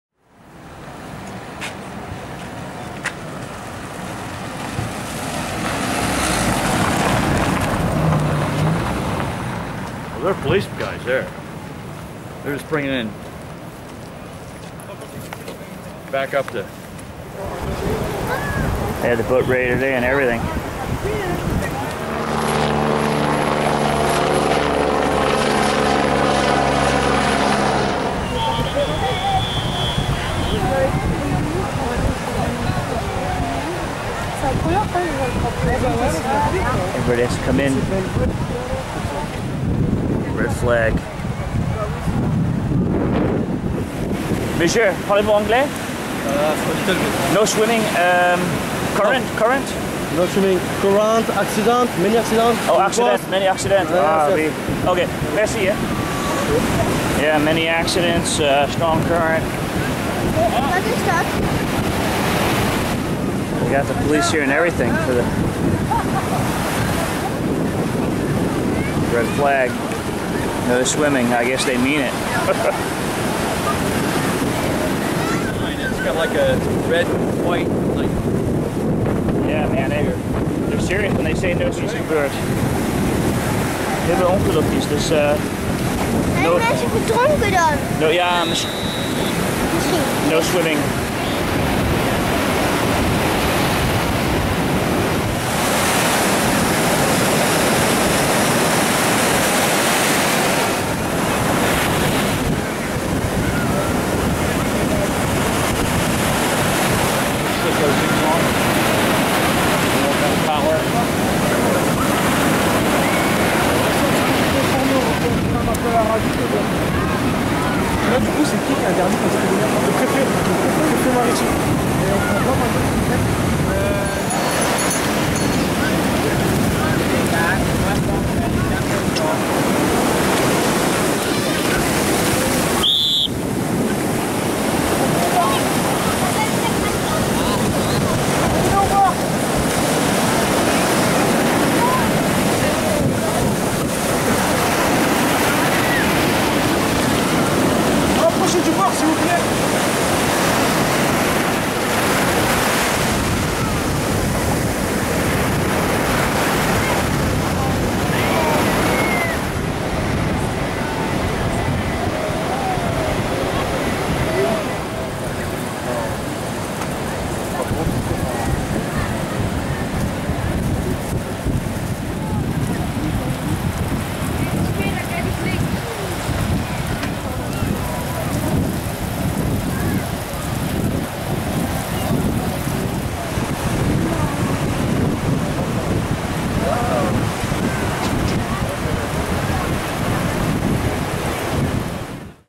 Marseillan Plage Code Red Rip Currents - aug 2014
Marseillan Plage Code Red Rip Currents. Red Flag, no swimming, dangerous rip tides or currents. Summer 2014. When the French say do not swim, they mean it. Police, life guards the works. And for good reason, it was reported two people died this day, on these very beaches. "a bright red flag, shaped as an isosceles triangle (base: 1.50 m; height : 2.25 m); this signal hoisted on the top of the mast meaning "bathing forbidden" "Marseillan Plage Code Red Rip Currents.